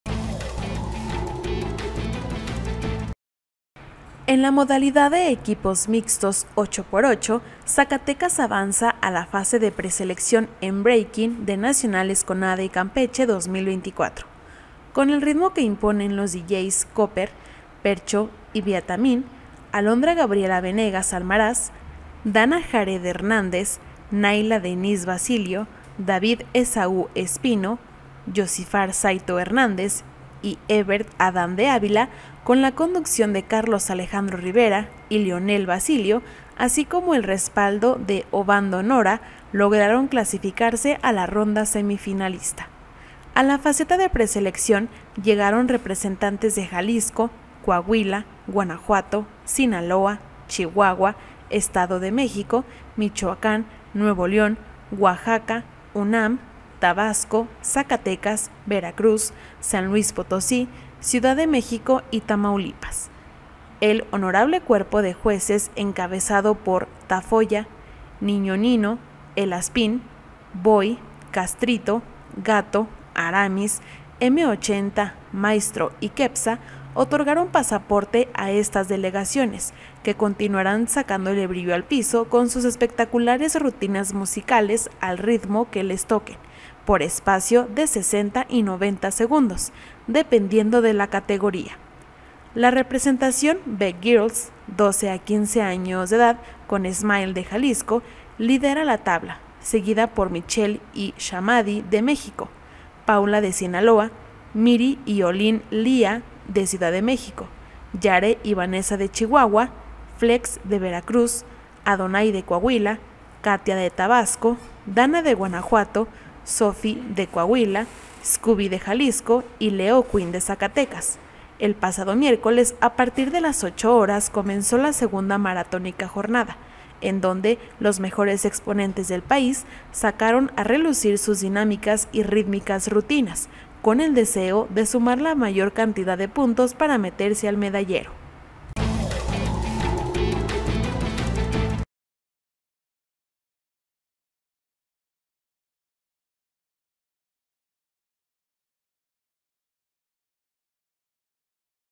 Zacatecas se situó en el Top 15 Preclasificatorio de Breaking en Nacionales CONADE Campeche 2024
En la modalidad de equipos mixtos 8x8, Zacatecas avanza a la fase de preselección en Breaking de Nacionales CONADE Campeche 2024. Con el ritmo que imponen los DJ Koper, Percho y Beatamine, Alondra Gabriela Venegas Almaraz, Danna Jareth Hernández Navarro, Nayla Denise Basilio Acosta, David Esaú Espino de Ávila, Josimar Saito Hernández y Everth Adán de Ávila Luna, con la conducción de Carlos Alejandro Rivera Martínez y Leonel Basilio Nava, así como el respaldo de Obando Nora Acosta lograron clasificarse a la ronda semifinalista. A la faceta de preselección llegaron representantes de Jalisco, Coahuila, Guanajuato, Sinaloa, Chihuahua, Estado de México, Michoacán, Nuevo León, Oaxaca, UNAM, Tabasco, Zacatecas, Veracruz, San Luis Potosí, Ciudad de México y Tamaulipas.